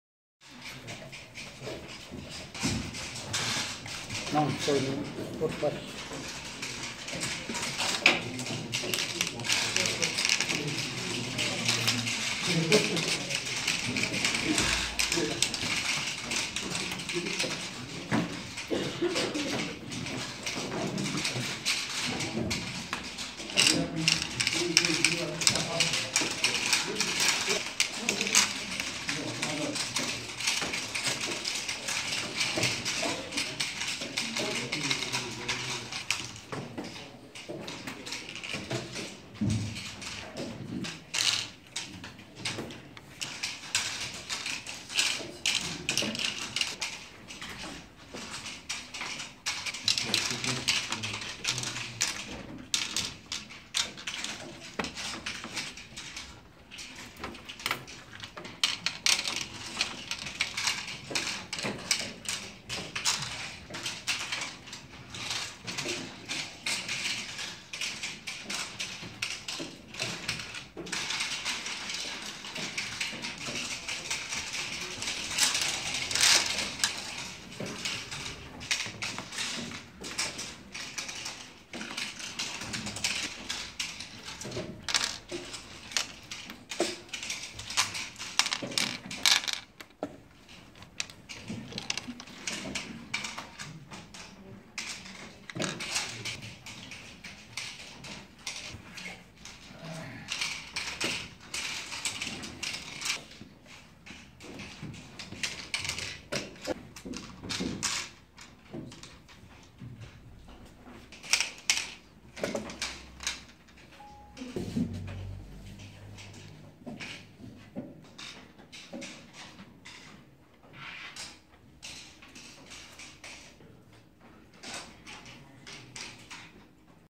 Тоғызқұмалақтан ҚР Ұлттық спорт түрлерінен V фестивалі (III - тур)
Тоғызқұмалақтан Қазақстан Республикасының Ұлттық спорт түрлерінен V фестивалі.
Ақтөбе қаласы,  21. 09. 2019 жыл